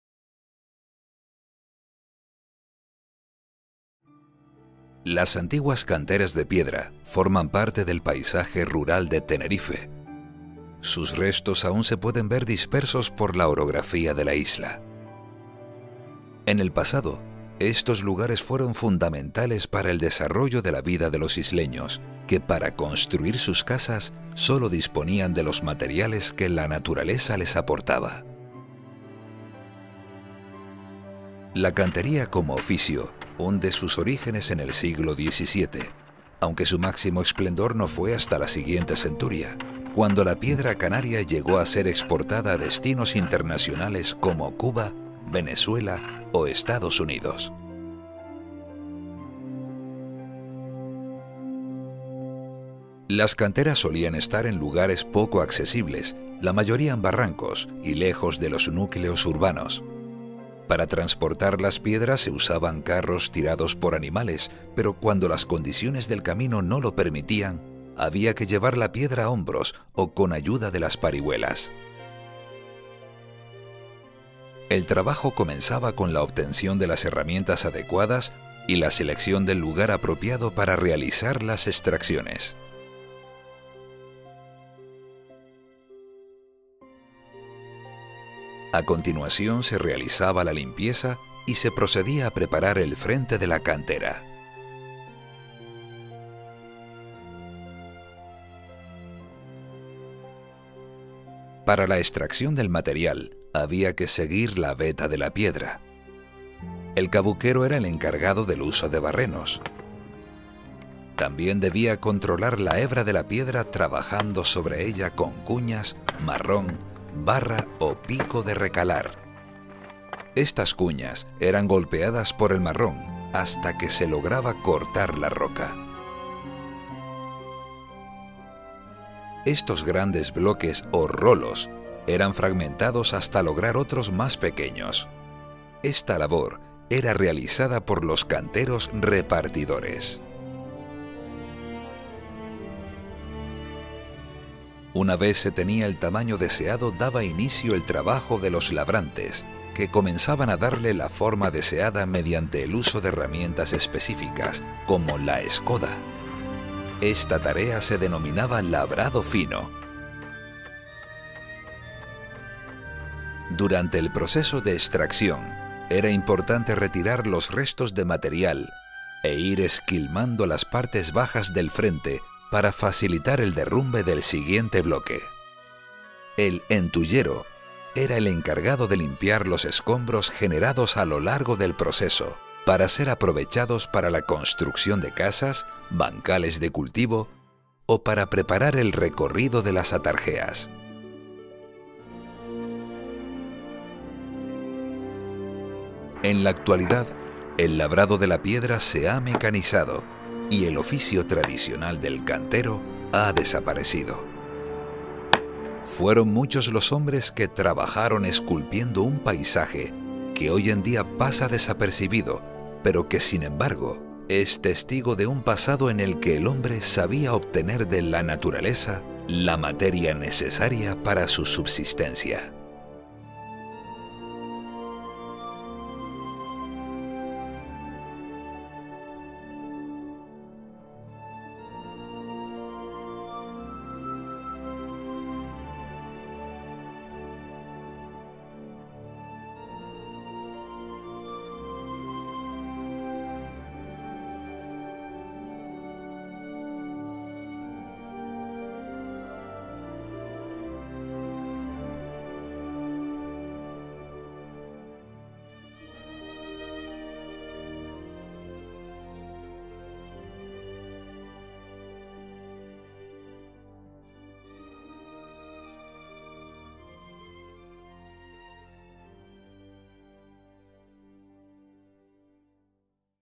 20170615_DO_Las canteras de piedra
El Cabildo, a través de la Fundación Tenerife Rural, quiere promover la recuperación del patrimonio etnográfico vinculado al mundo rural a través de varios audiovisuales que pongan en valor diferentes tradiciones y modos de vida de antaño.